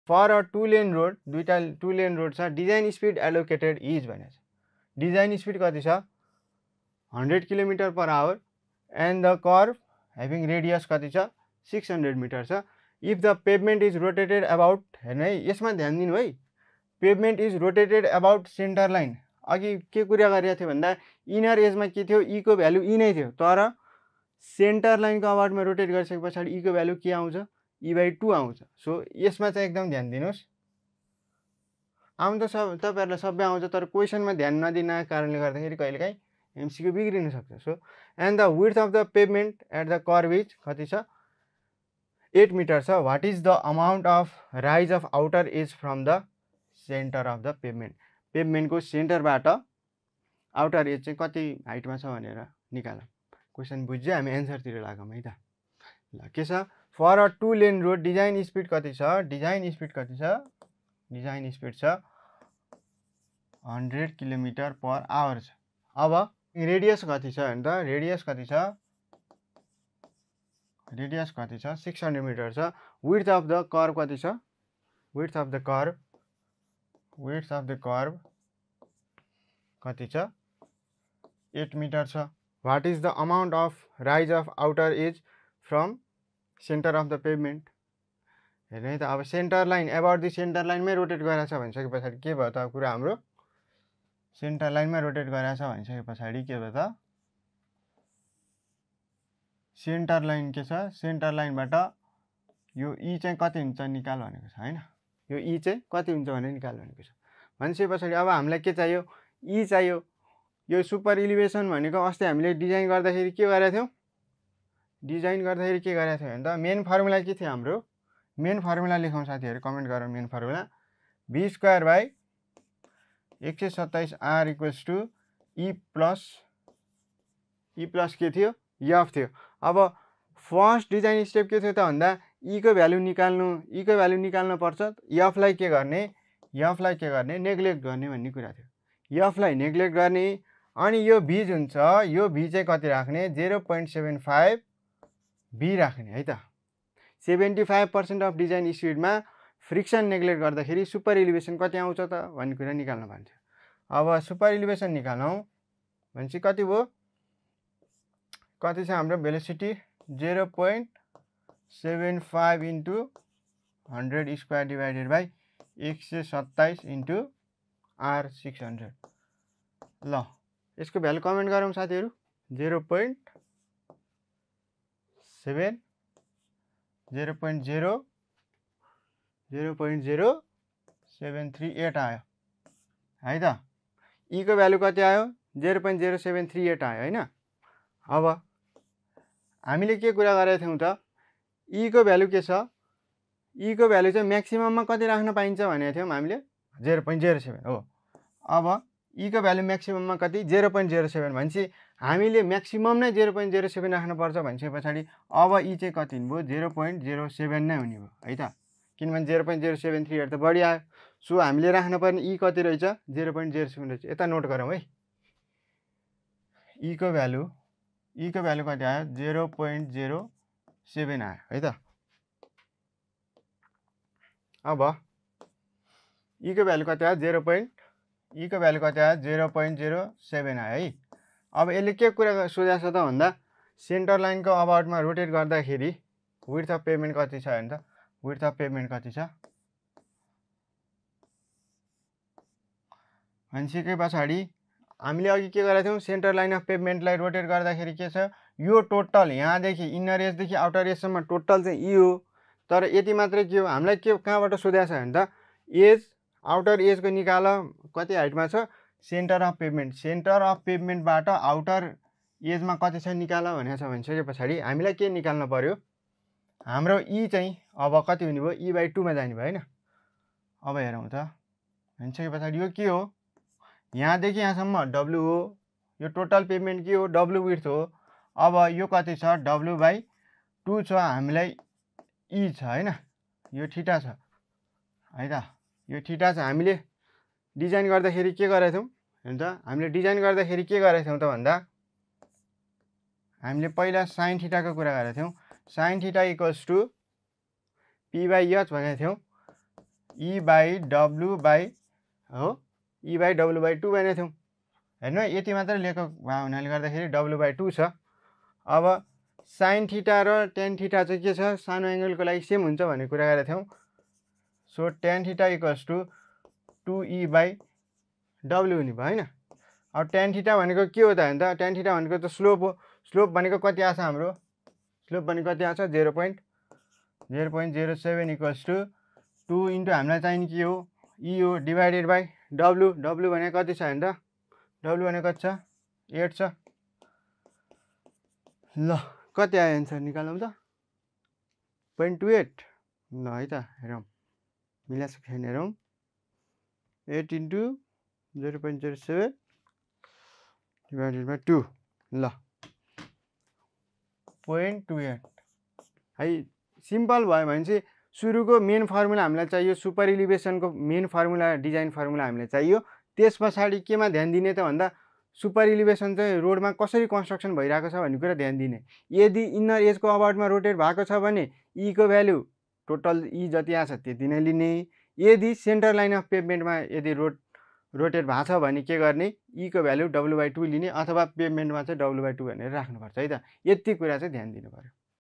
Super elevation MCQ | Geometric Design of highway
In this video our guru Saroj Pandit is solving and explaining the conceptual  MCQ regarding transportation engineering.